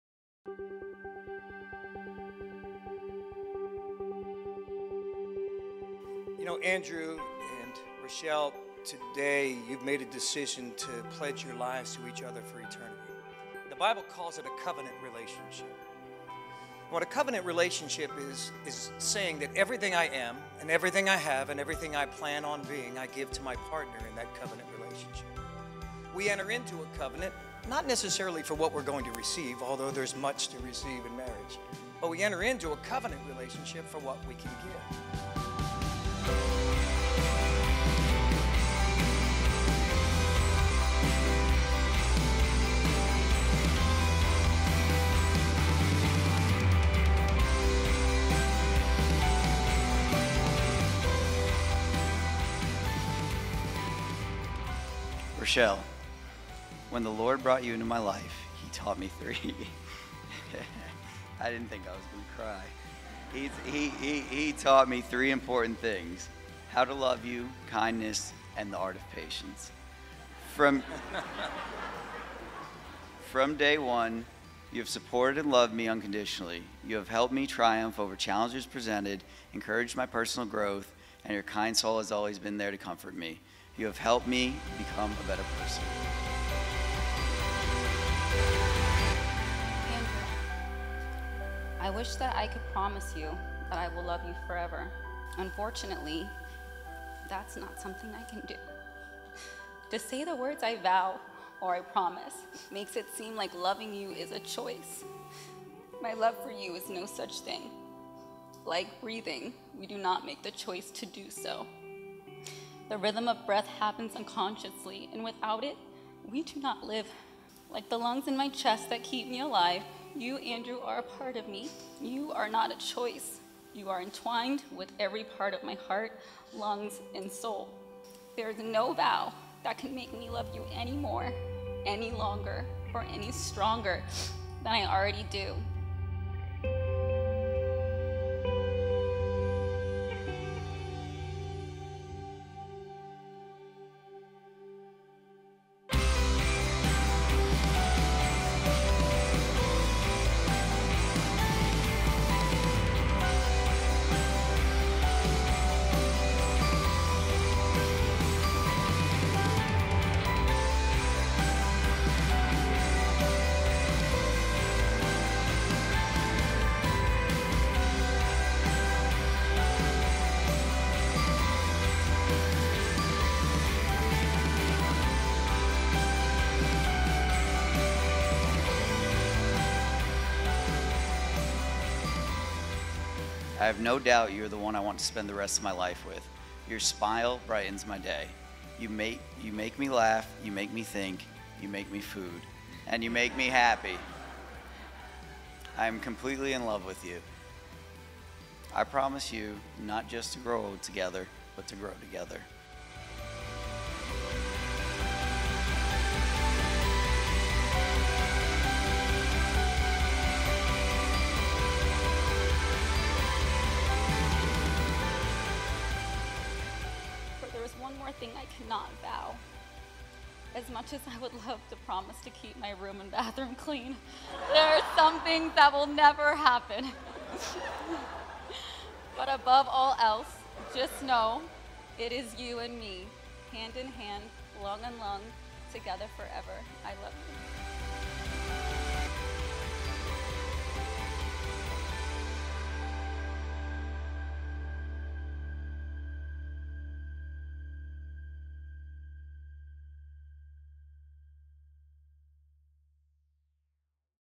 Wedding Trailer -- A look back at our wedding at The Breakers Palm Beach
September 23, 2017 I married my soul mate. Take a look back with us as we celebrate our first year wedding anniversary!

Videographer: Merge Social
Venue: The Breakers Palm Beach